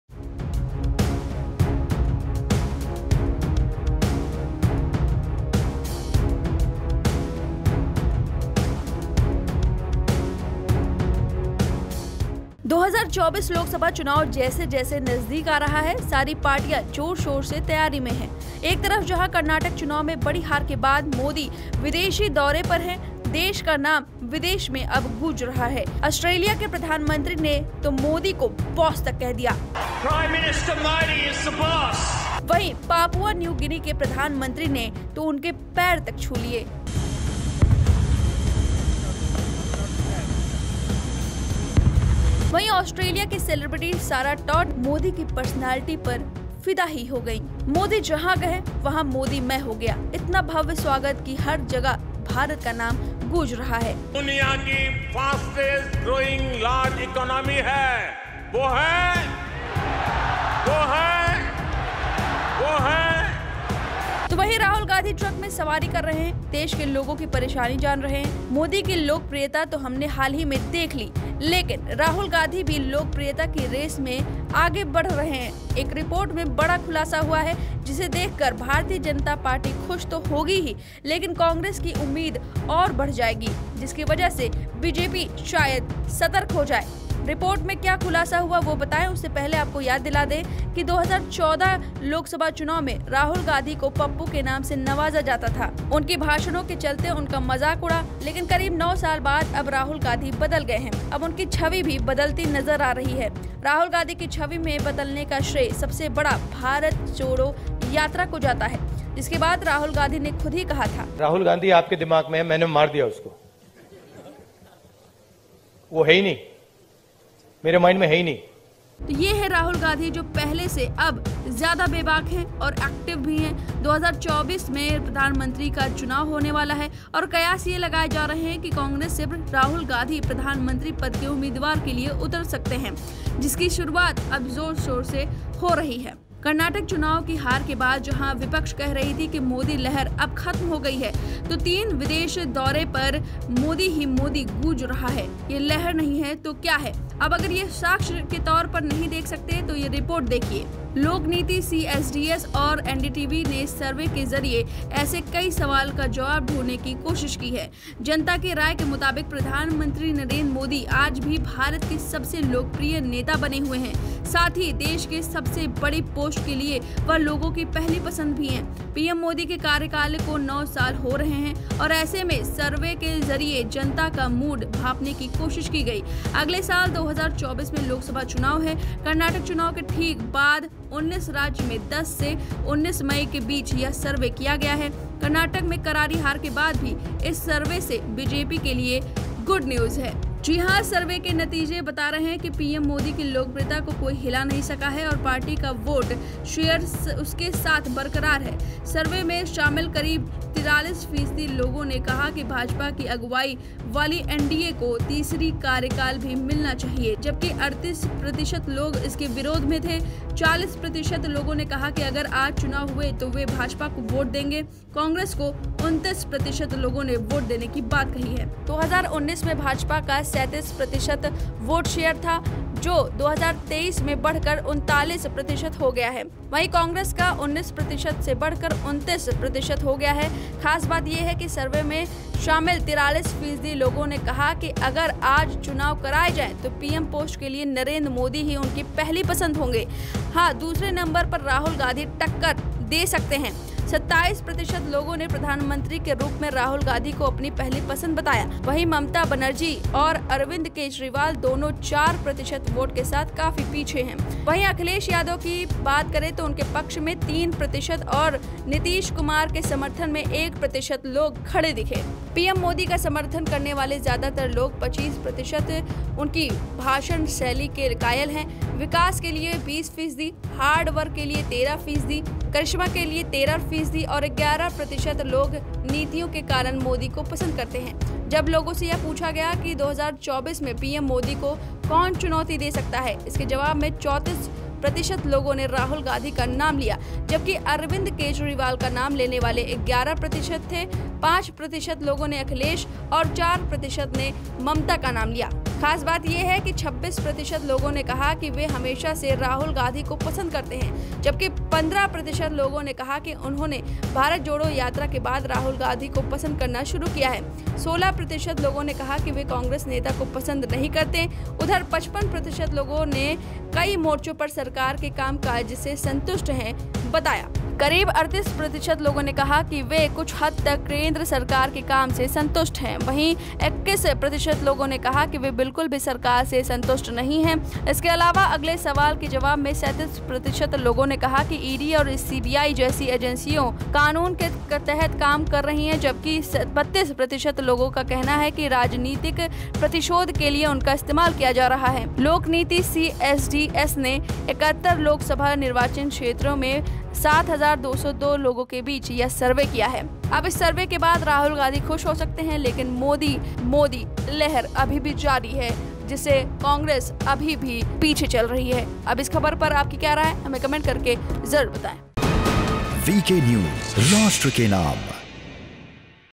पीएम मोदी और राहुल गांधी में से कौन हैं नंबर वन ? PM Modi VS Rahul Gandhi
2024 लोकसभा चुनाव जैसे जैसे नजदीक आ रहा है.. सारि पार्टियां जोर शोर से तैयारी में हैं.. एकतरफ जहाँ कर्नाटक चुनाव में बड़ी हार के बाद मोदी विदेशी दौरे पर हैं.देश का नाम विदेश में  गूँज   रहा है.ऑस्ट्रेलिया के प्रधानमंत्री ने मोदी को बॉस कह दिया ( मोदी इस बोस) .. वहीँ पापुआ न्यू गिनी के प्रधानमंत्री ने... उनके पैर तक छू लिए... वहीँ ऑस्ट्रेलिया की सेलेब्रिटी सारा टॉड मोदी की पर्सनालिटी पर फ़िदा हो गयी। .. मोदी जहाँ गए वहां मोदी मई हो गया... इतना भव्य स्वागत की हर जगह भारत का नाम गूँज रहा... ( मोदी भाषण वो कौन देश हैं)
As 2024 Loksabha election is coming nearer..all the parties are preparing loudly..on the one hand where Modi is on foreign tour after big defeat in karnataka election.the name of the country is echoing abroad.Prime Minister of Australia Modi Called Ko Boss (Modi is Bose) .. Whereas the Prime Minister of Papua New Guinea... touched his feet... Wherein Australia's celebrity Sara Todd fell in love with Modi's personality. ..Wherever Modi went, there was Modi May… Such a grand reception, the name of India echoed everywhere… (Modi speech, which country is that)